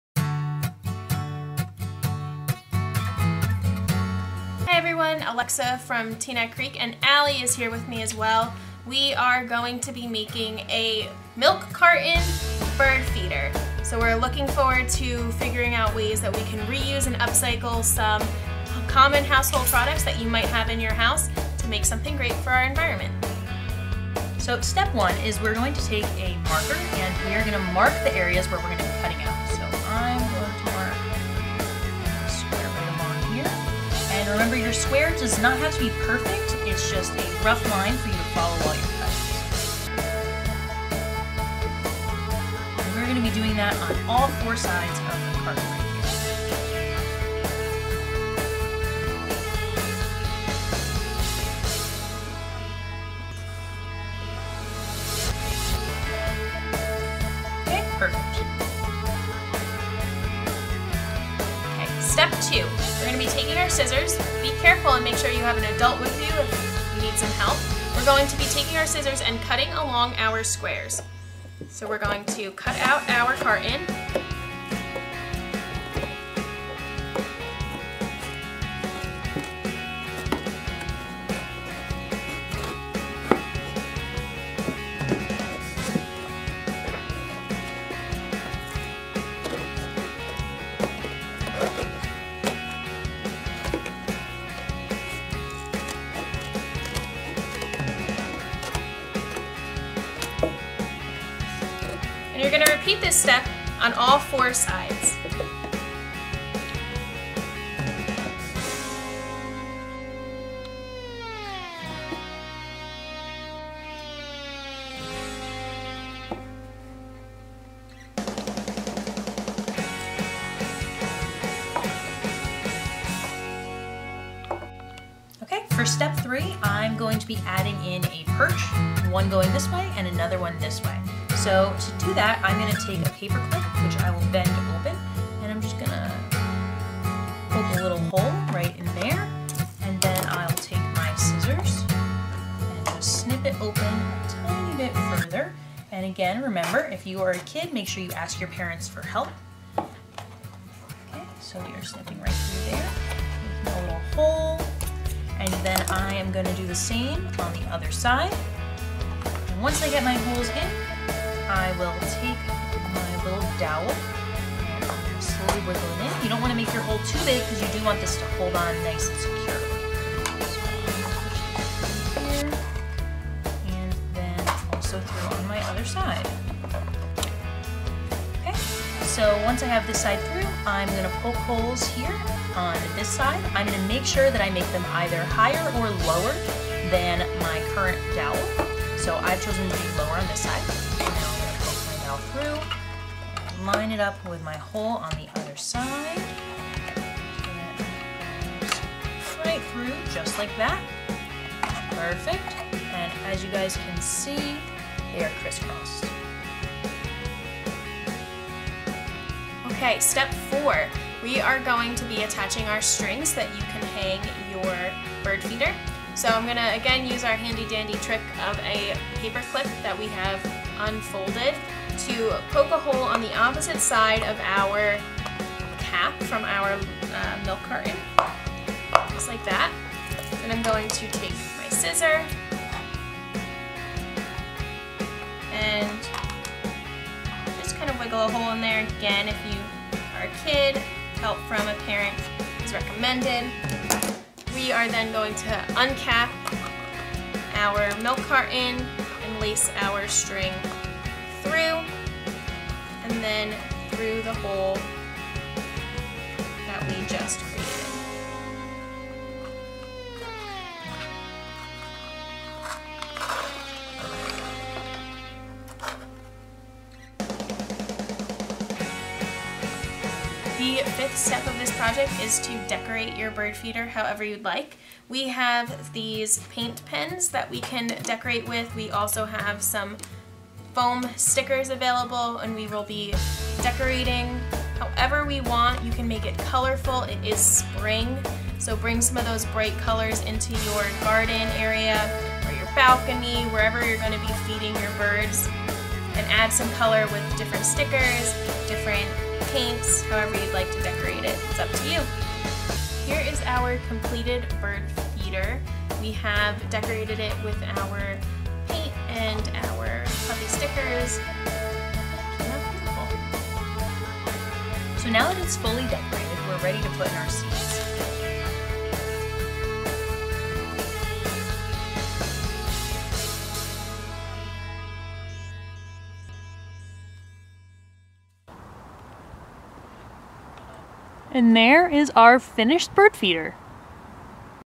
Milk Carton Bird Feeder
Join Alexa and Allie for this kid-friendly Crafternoon project, a milk carton bird feeder! This easy quarantine project reuses waste to create a colorful feeder for migrating birds.